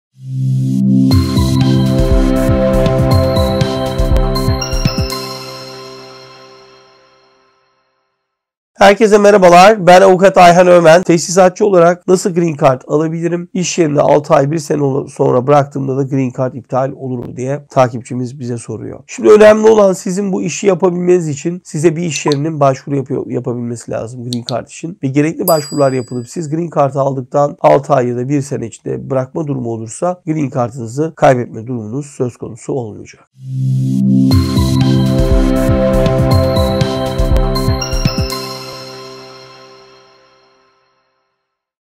TESİSATÇI OLARAK AMERİKA’DA ÇALIŞIP NASIL GREEN CARD ALIRIM ?
Soru: Tesisatçı olarak nasıl Green Card alabilirim ? İşyerini 6 ay 1 sene sonra bıraktığımda da Green Card iptal olur mu ?